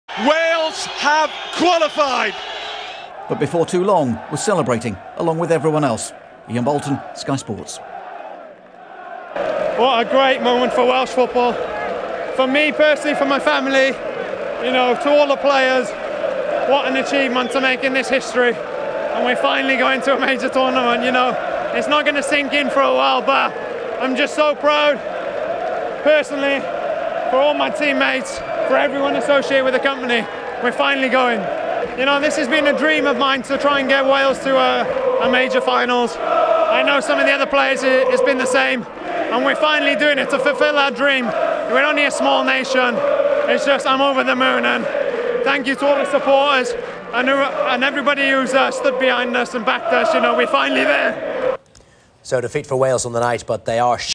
Aaron Ramsey interview after Wales qualify for Euro 2016 | Euro 2016 Qualifiers
Aaron Ramsey on what it means to Welsh football